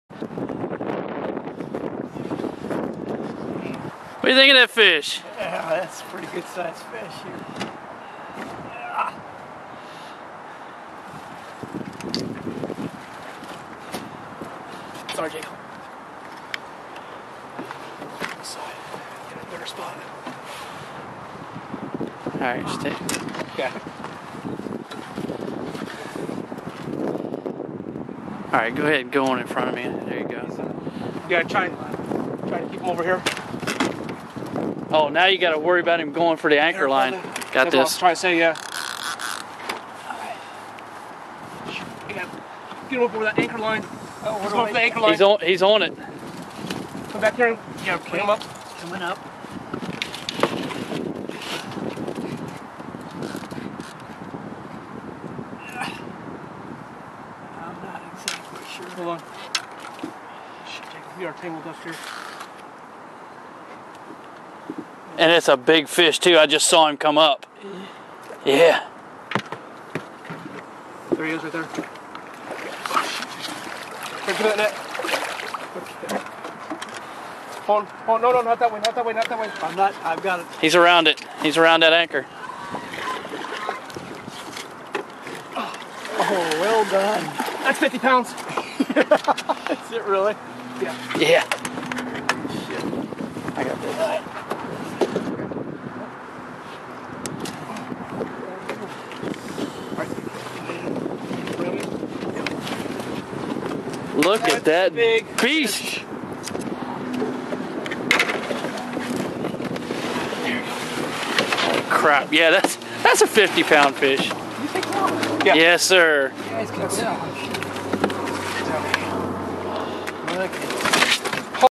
catfishing rookie fighting a big blue.MP4
First time catfisherman fighting his biggest fish of the day, a nice 42.6 pounder on the Potomac River